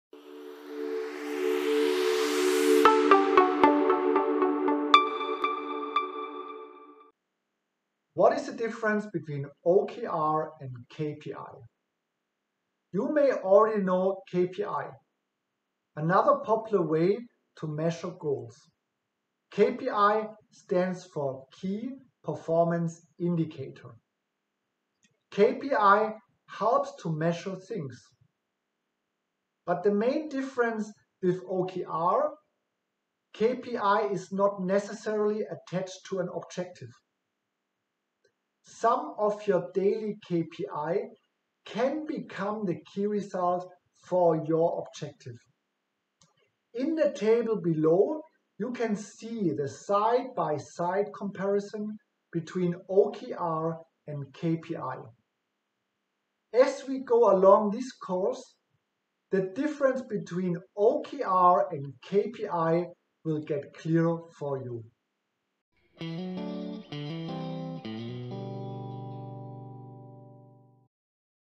What is the Difference between OKR and KPI?
This video is part of the OKR Institute's OKR Foundation Course. Enroll to the full course and get your certificate

This course is facilitated by Dirk Schmellenkamp, OKR and KPI Coach, who supports individuals and teams around the globe to implement OKR successfully in their companies.